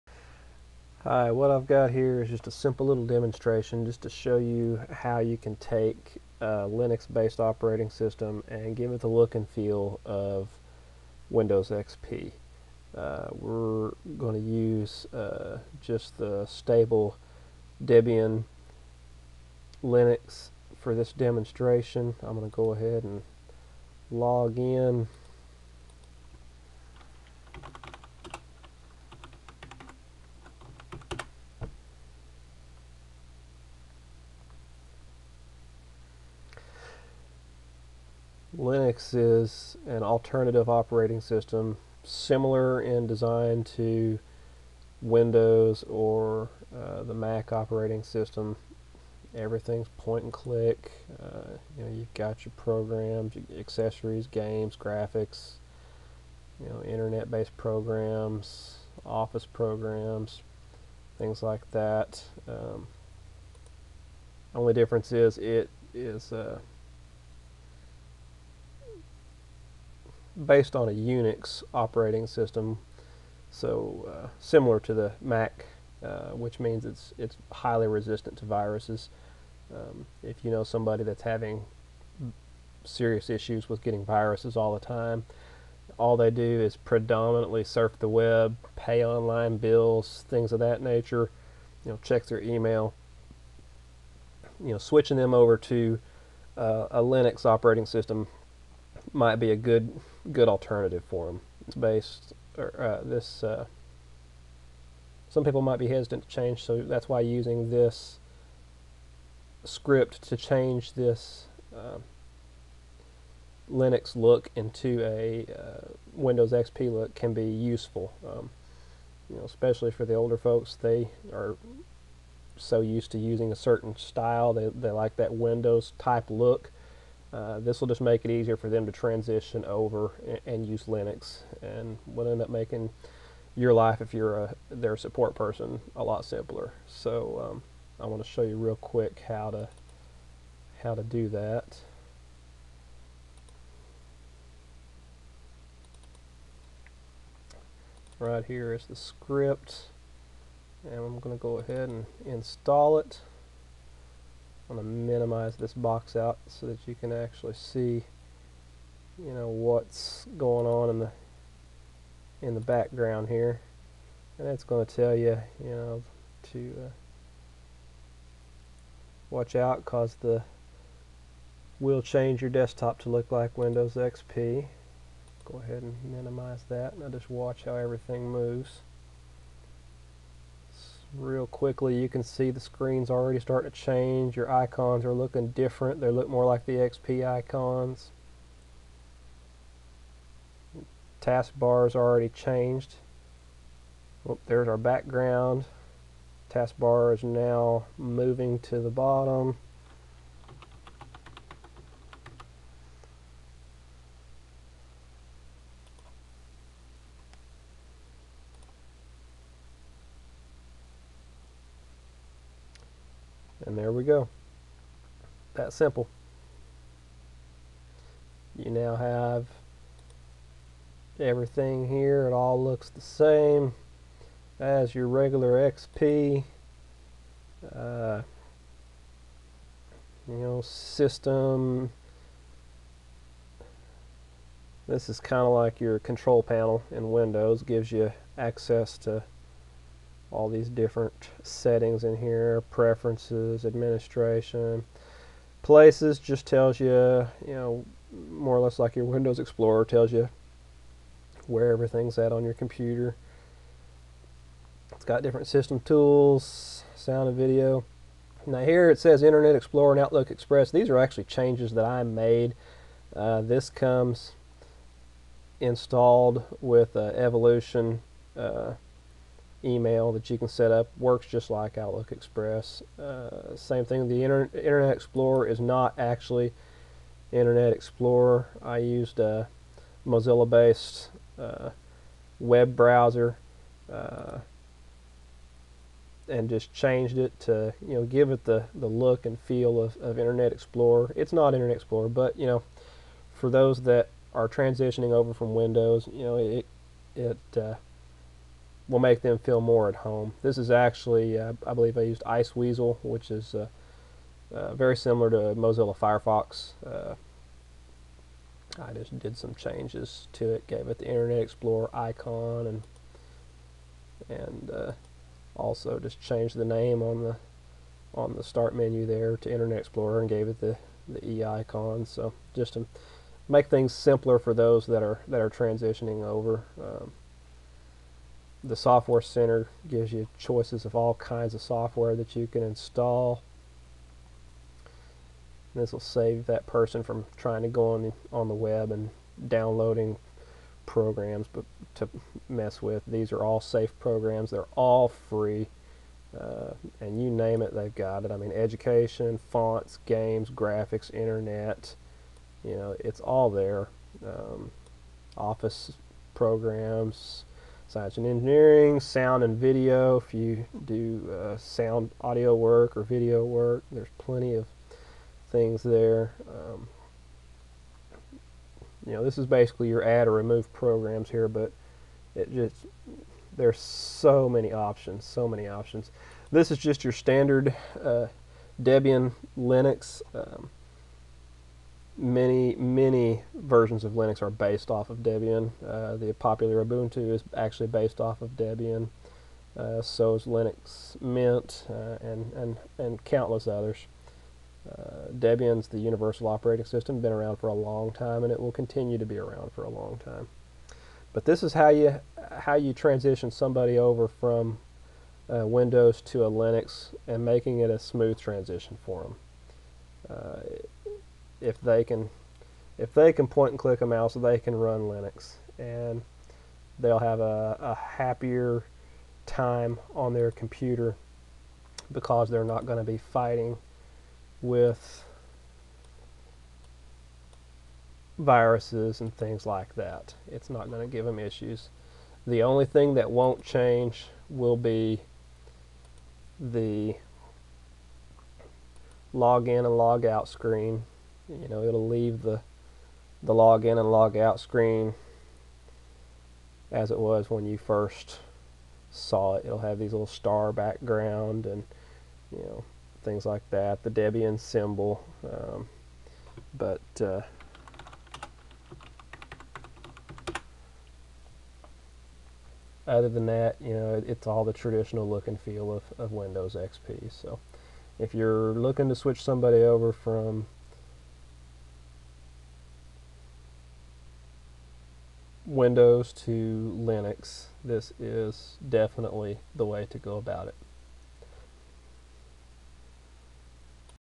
Easily transition someone from Windows to Linux by making Linux look like Windows XP
Easily transition someone from Windows to Linux by making Linux look like Windows XP.  This video shows you how to make Debian Linux look like Windows XP.